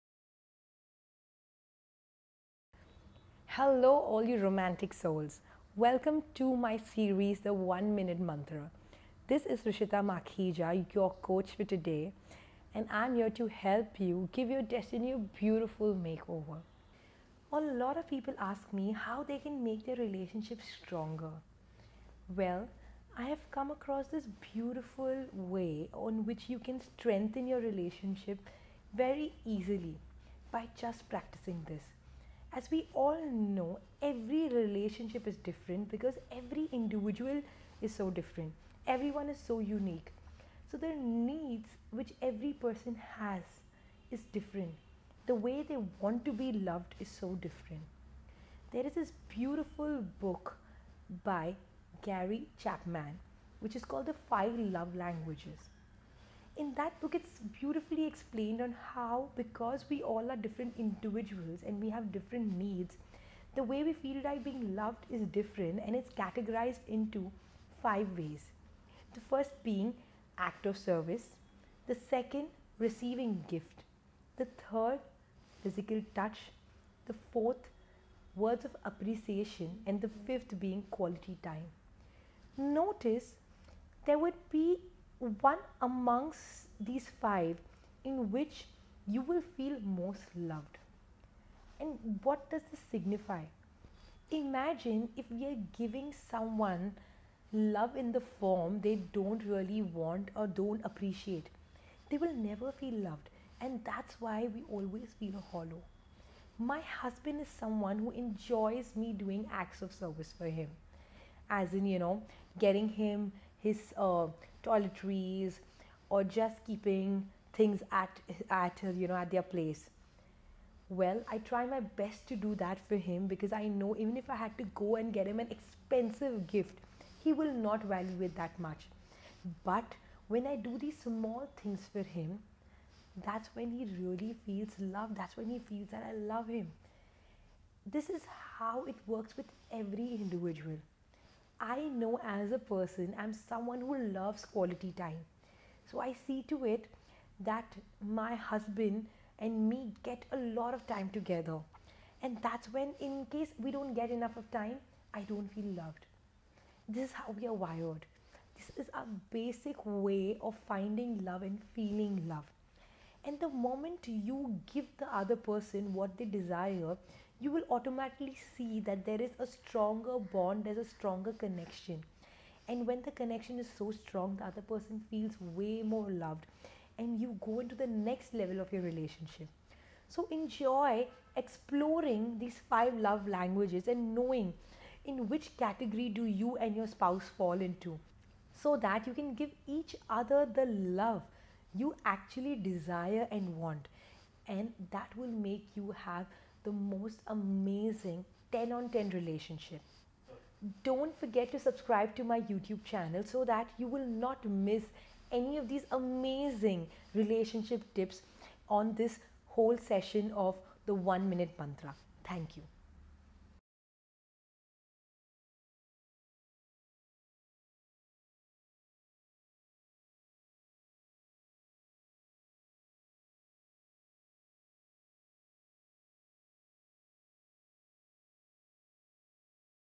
Effective mantras to make your relationship stronger.
Relationships are not easy to maintain it takes a lot of efforts, and when these efforts go down in the drain it hurts the most. So follow and implement these tips in your relationship and make it strong like a diamond!

Much Love and Gratitude,
Rishita Makhija